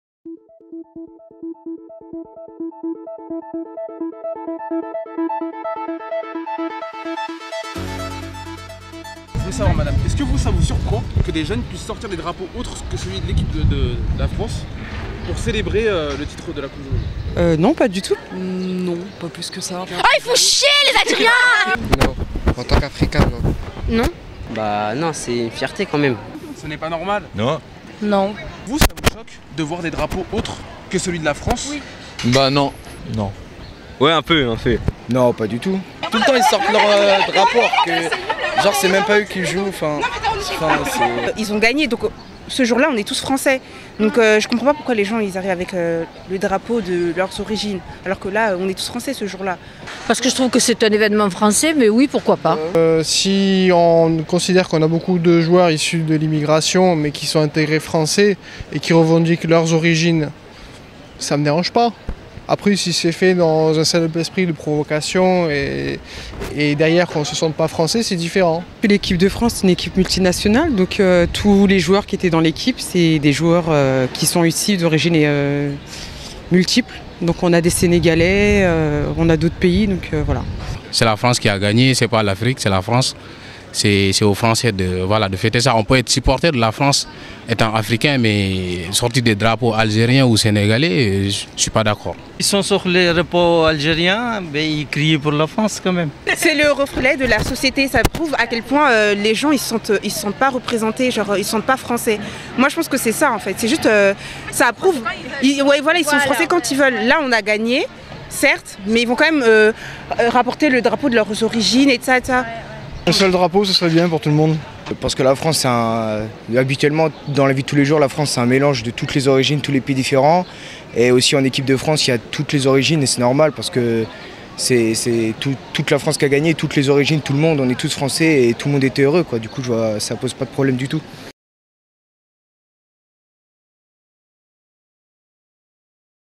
Drapeaux étrangers pour fêter la victoire des Bleus au Mondial : choquant ou pas ?
La caméra d’Oumma a posé la question, objet de polémiques passionnelles, à des passants en région parisienne. Découvrez leurs avis partagés et totalement dépassionnés !